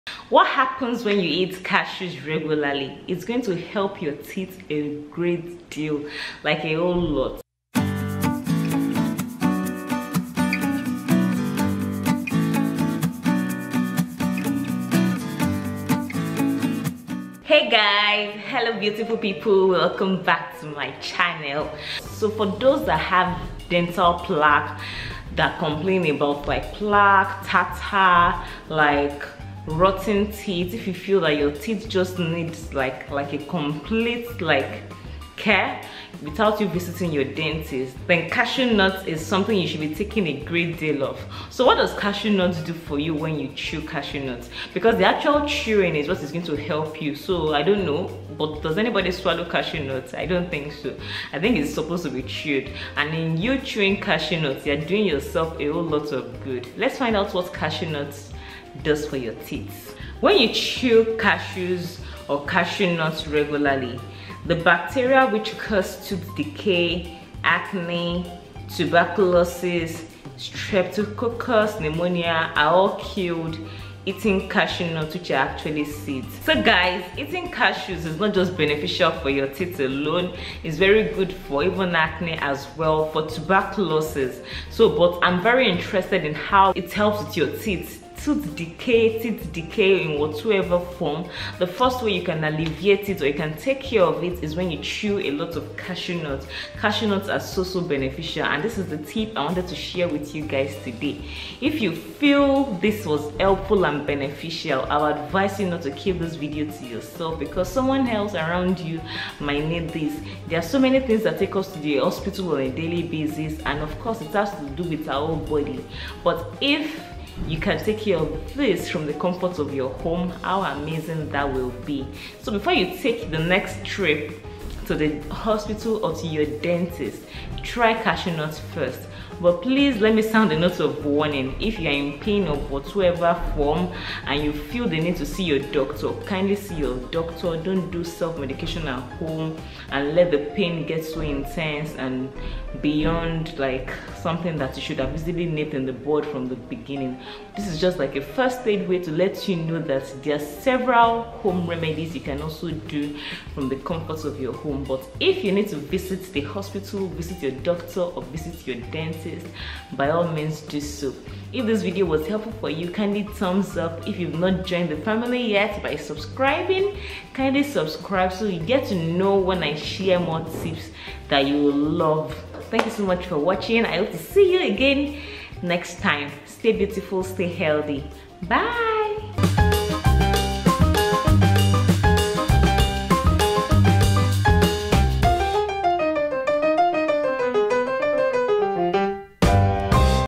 What Will Happen to Your Body If You Eat 4 Cashews Every Day
Cashew nuts are a great source of copper and antioxidants that are both beneficial for your skin. Antioxidants have been known to reduce the signs of aging, and copper, along with other enzymes, produces the collagen that is responsible for increasing the elasticity of your skin.